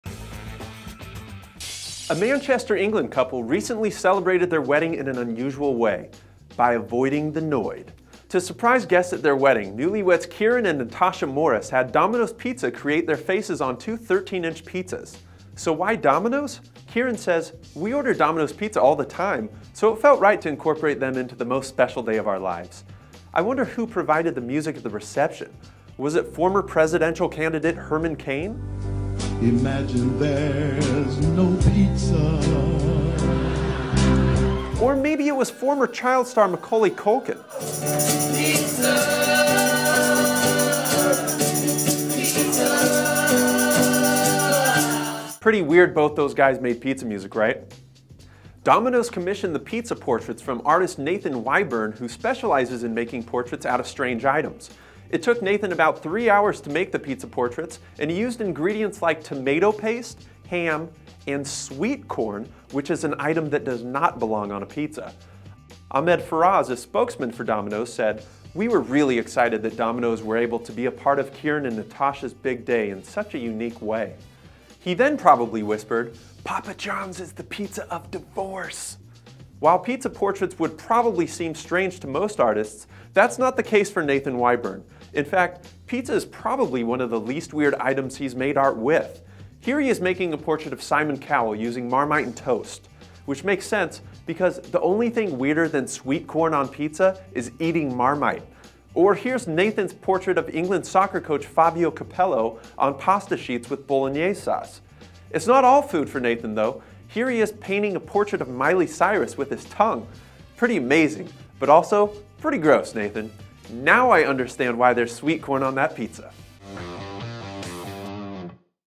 Newlyweds Enjoy Custom Pizza Portraits From Food Artist
To surprise guests at their wedding, Kieran and Natasha Morris had Domino's pizza create their faces on two 13-inch pizzas. Domino's commissioned the pizza portraits from artist Nathan Wyburn, who specializes in making portraits out of strange items like toast and pasta. It took Nathan about 3 hours to make the pizza portraits, and he used ingredients like tomato paste, ham, and sweet corn. Cheers!